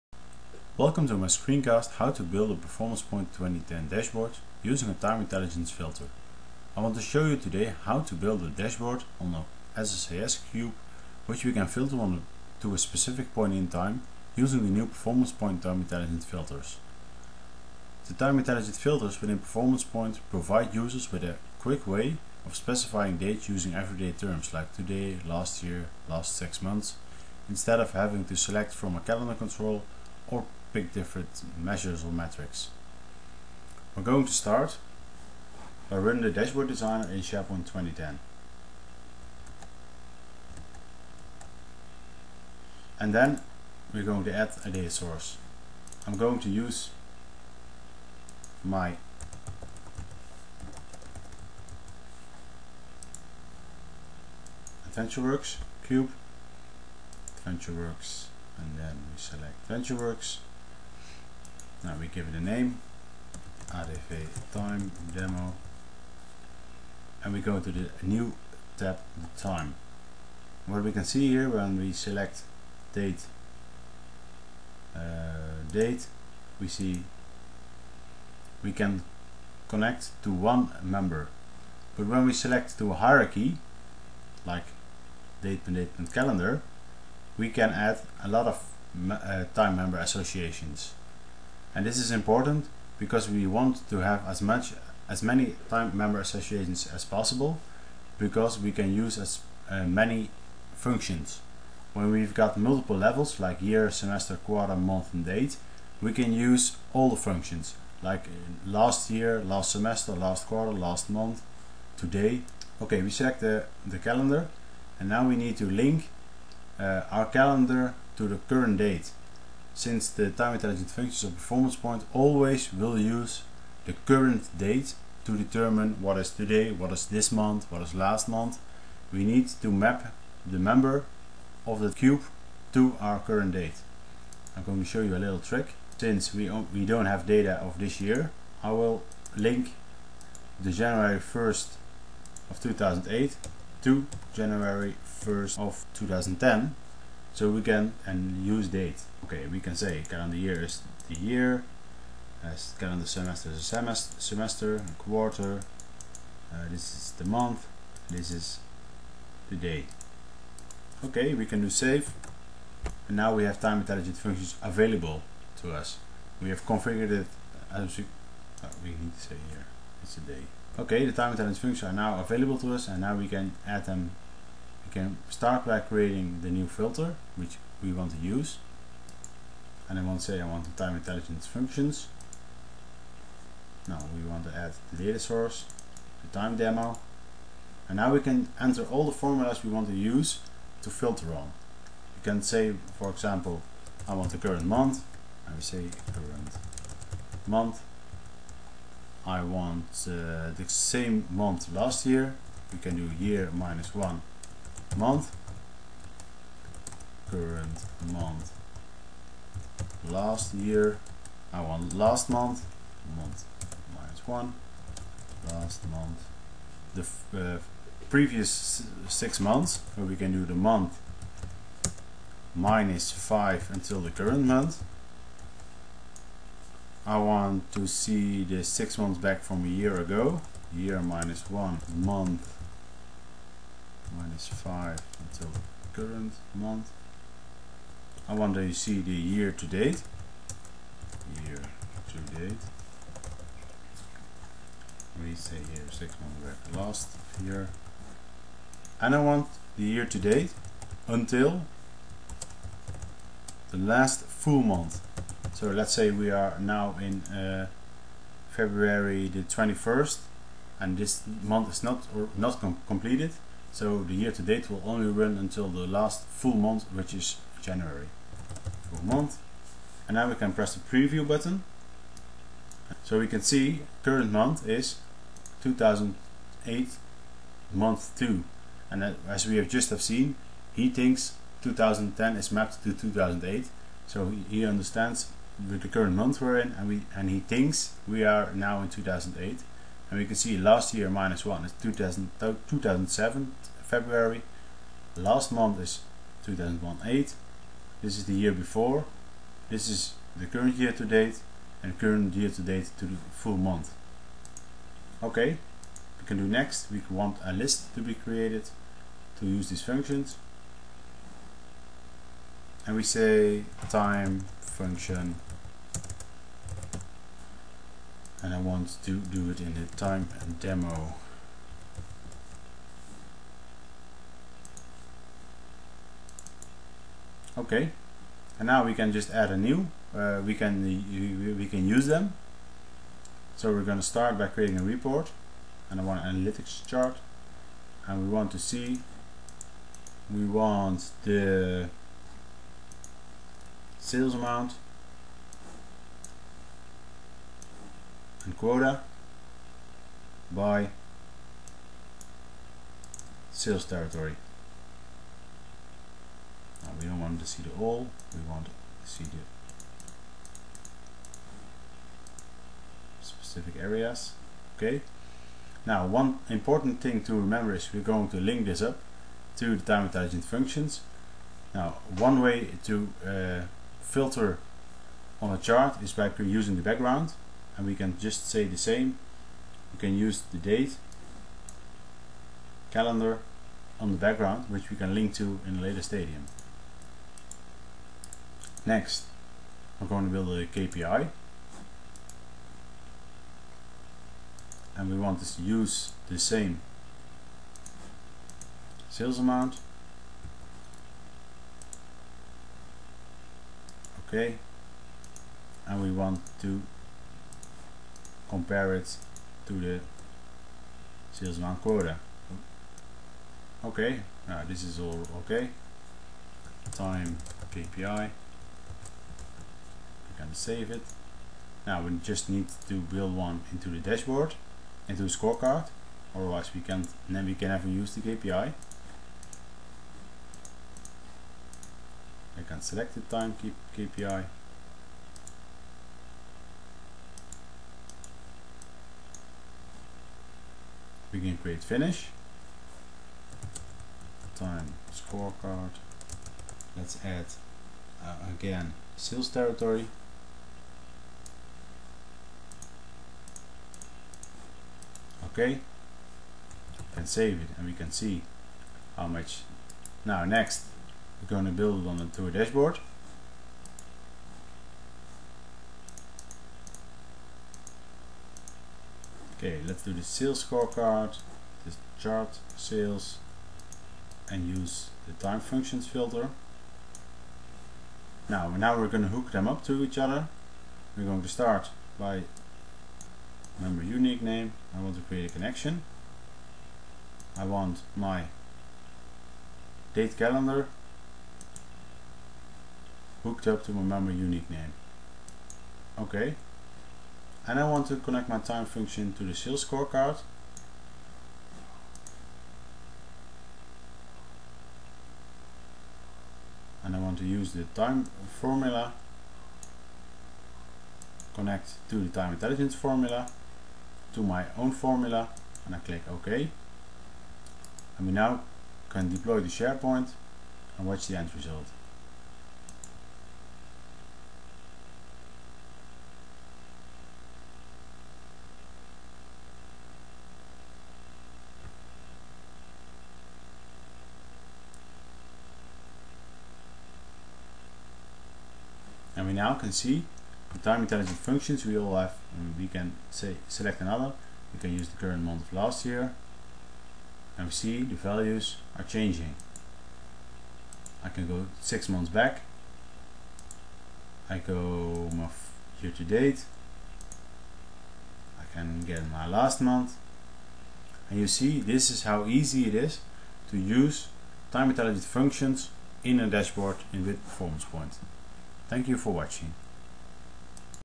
Build a PerformancePoint 2010 dashboard using a Time Intelligence filter
This screencast will show you how we create a dashboard on a SSAS cube in PerformancePoint 2010 using a filter with time intelligent functions.